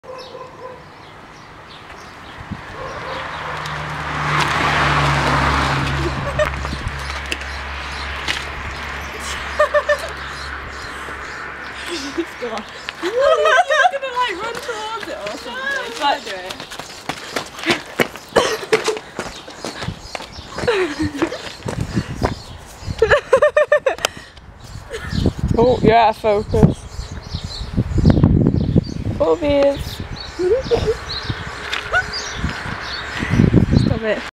Crime thriller blooper
In this scene I was supposed to be giving Jade (Bryony Warren) a 'secret envelope' containing 'secret stuff' ... it somehow turned into this... Enjoy! :P